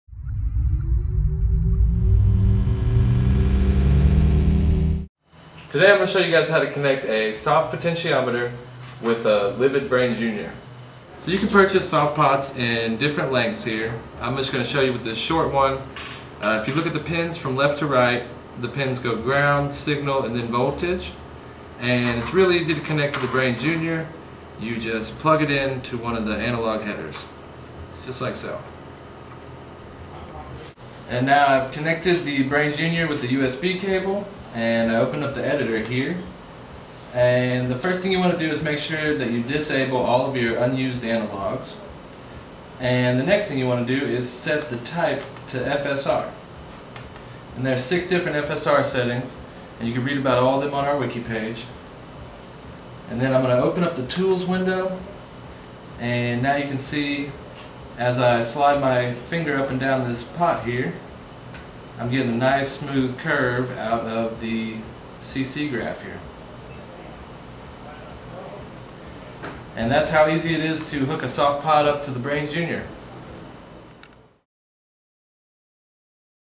Brain Jr with a Soft Potentiometer
Connecting a soft potentiometer with the Brain jr.